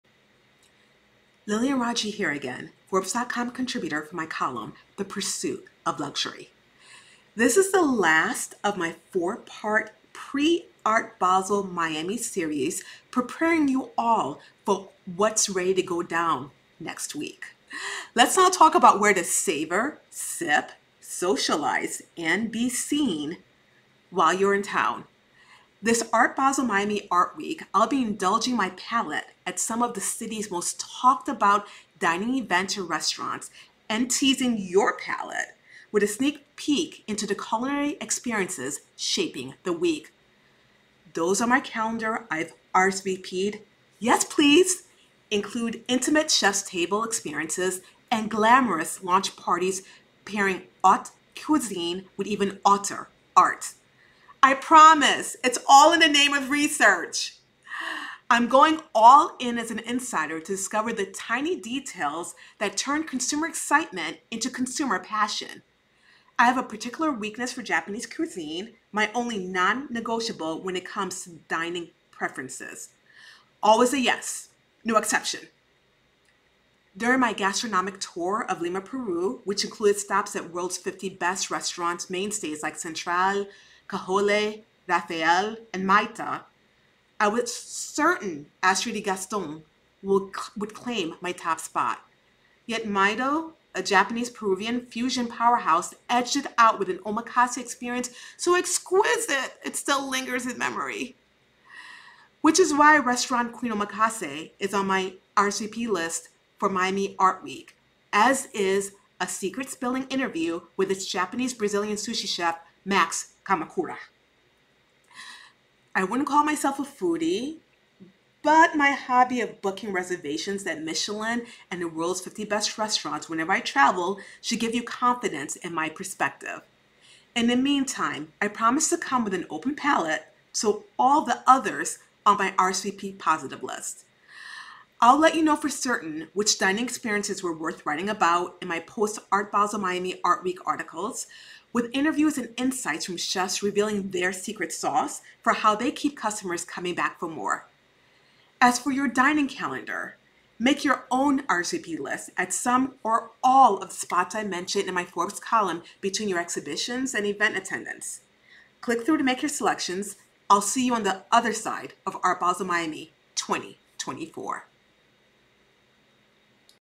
Where to Savor, Sip, and Socialize During Art Basel Miami Art Week 2024
Art Basel Miami Art Week delivers culinary magic to fuel conversations, connections, and celebrations in between examining artistic interpretations of, say, a banana. This year, the city’s top chefs, mixologists, and restaurateurs are crafting unforgettable dining experiences to rival the art on display.

From Japanese omakase at Queen Miami to the Mediterranean-Latin fusion of CASA NEOS, each destination promises an immersion into the flavors and stories shaping Miami’s delightful gastronomic scene. Highlights this year include:
🍣 Queen Omakase: An exclusive eight-seat dining journey led by Chef Max Kamakura, blending Japanese tradition with modernist flair.
🍸 Panamericano Bar: A speakeasy gem hidden in Mary Brickell Village, featuring Latin American-inspired cocktails and an exclusive bartender pop-up.
🎨 Barcelona Wine Bar: A live art happy hour with Patrick Church, merging creativity and cuisine in the heart of Wynwood.
🍷 Delilah Miami: A Jazz Age-inspired supper club offering live performances and a star-studded brunch experience.

Whether you’re indulging in MILA’s Balmain takeover or mingling at R House Wynwood’s activist art exhibit, these venues will be a feast for all your senses.
🍴 Where will you dine between exhibits and parties?
🍹 Which events offer the perfect pairing of art and cuisine?
🎉 What makes these culinary experiences stand out during Art Basel Miami Art Week?

Get the full list of can’t-miss dining destinations. Read the full article for all the details.